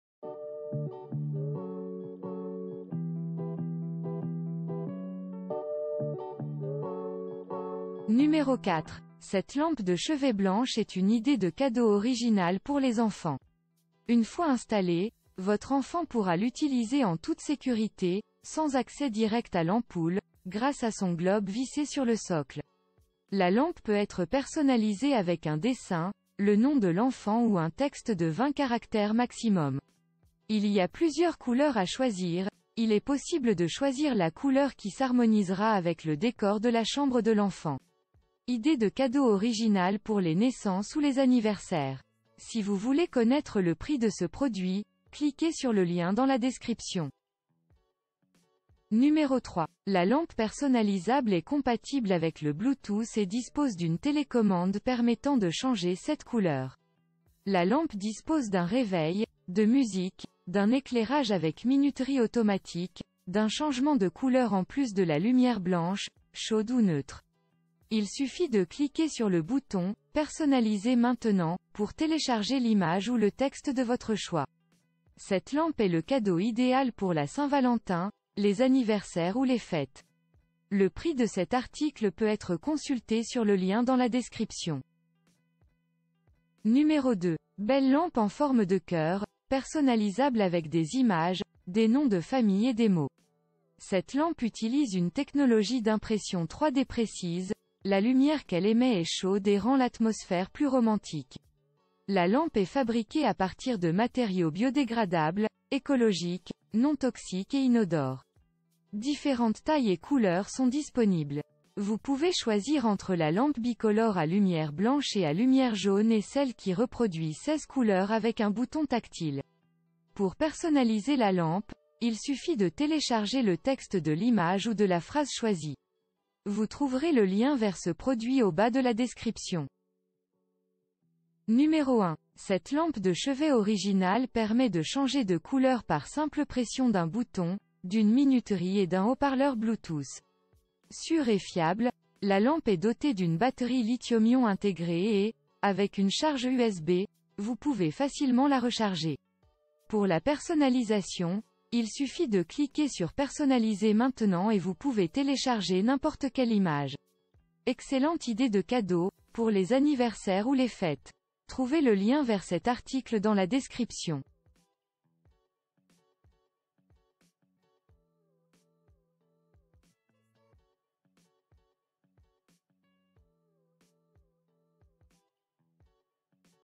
Les 4 meilleures lampes de chevet personnalisables
1) Hjsadgasd - Haut-parleur ... »
2) Baoshou - Image Personnal... »
3) Asd Jewerly - Personnalis... »
4) Lifeislikearainbow - Life... »

✋ Voici les 4 meilleures lampes de chevet personnalisables, disponibles sur le marché.

00:00 - Introduction
00:08 - 4) Lifeislikearainbow - Life is like a rainbow - Lampe de cheve...
00:48 - 3) Asd Jewerly - Personnalisé photo personnalisé 3d lampe photo...
01:26 - 2) Baoshou - Image PersonnaliséE Lampe De Chevet Moon Light, Te...
02:11 - 1) Hjsadgasd - Haut-parleur Bluetooth à lumière nocturne à LED ...